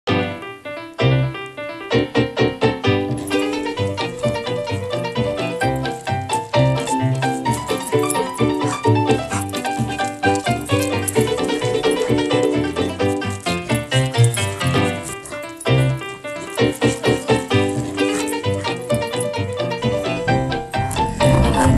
Saint Bernard on Ice
Our dog loves ice !!! Who knew ?